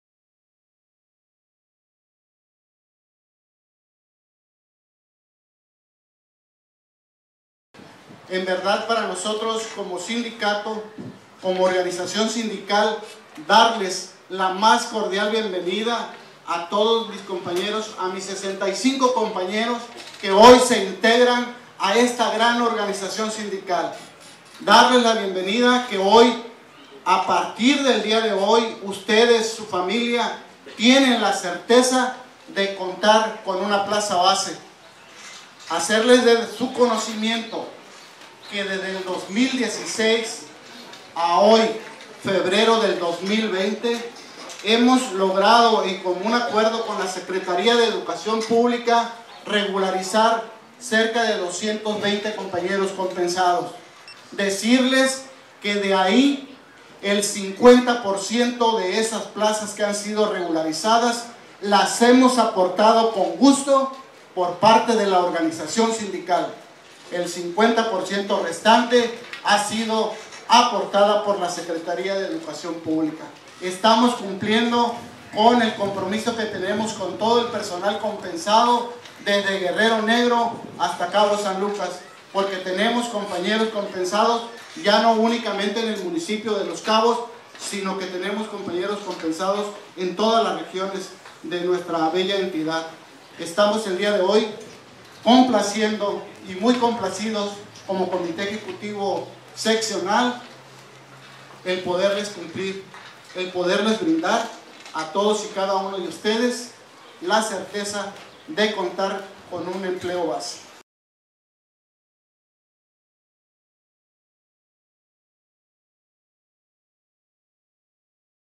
Prof. Nicolás Gutiérrez, evento público de entrega de 65 plazas para Personal de Apoyo y Asistencia
Mensaje del profesor José Nicolás Gutiérrez Domínguez, secretario general de la Sección 3 del SNTE en B. C. Sur, durante el evento público de entrega de 65 plazas para Personal de Apoyo y Asistencia a la Educación, realizado el jueves 6 de febrero del año en curso, en la Secretaría de Educación Pública en el evento.